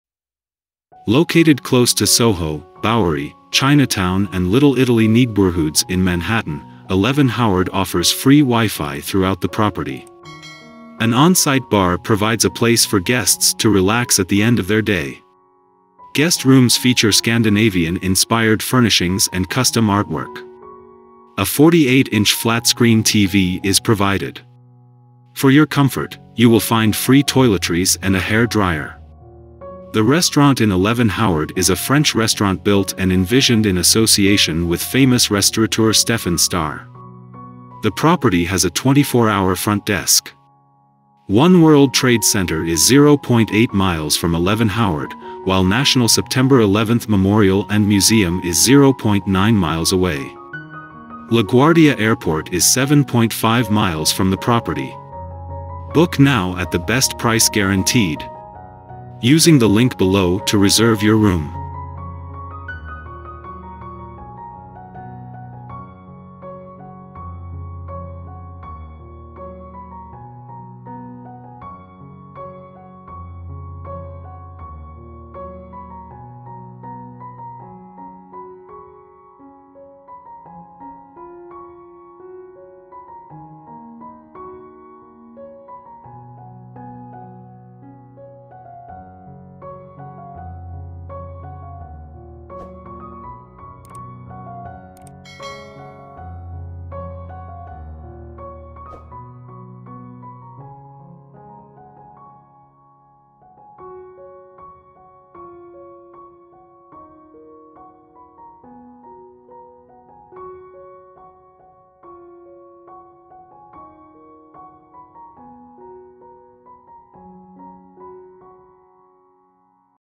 11 Howard
Book now

Located close to Soho, Bowery, Chinatown and Little Italy neigbourhoods in Manhattan, 11 Howard offers free WiFi throughout the property. An on-site bar provides a place for guests to relax at the end of their day.
Guest rooms feature Scandinavian inspired furnishings and custom artwork. A 48-inch flat-screen TV is provided. For your comfort, you will find free toiletries and a hair dryer.

The restaurant in 11 Howard is a French restaurant built and envisioned in association with famous restaurateur Stephan Starr. The property has a 24-hour front desk.

One World Trade Center is 0.8 mi from 11 Howard, while National September 11 Memorial & Museum is 0.9 mi away. La Guardia Airport is 7.5 mi from the property.